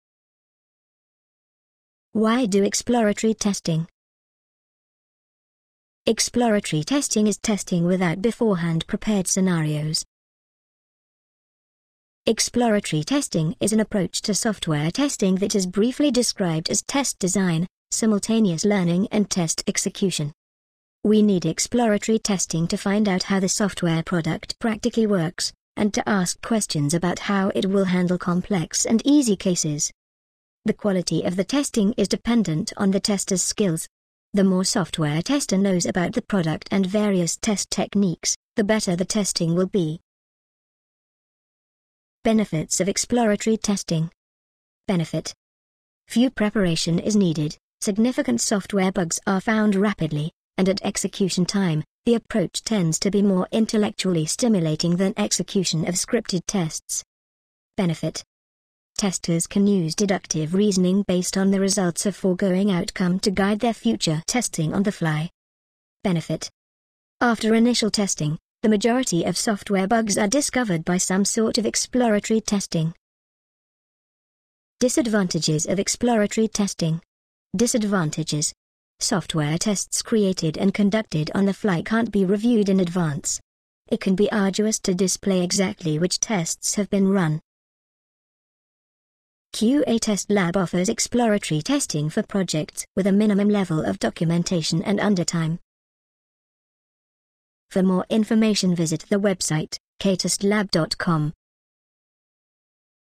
Why Do Exploratory Testing?
Exploratory testing is testing without beforehand prepared scenarios. Exploratory testing is an approach to software testing that is briefly described as test design, simultaneous learning and test execution.